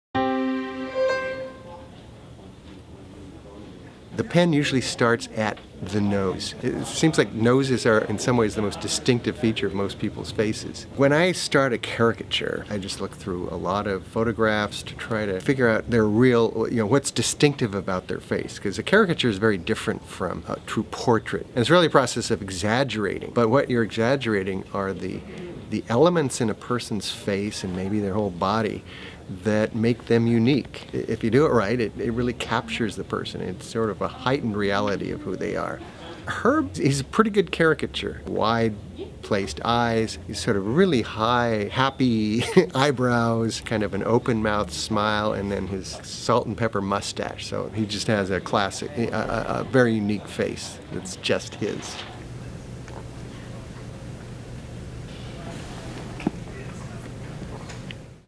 Inside the Head of a Political Cartoonist
David Horsey, political commentator and cartoonist for the Los Angeles Times, invites us into his process for drawing personalities.

"It's really a process of exaggerating, but what you're exaggerating are the elements in a person's face or maybe their whole body, that make them unique." ~ David Horsey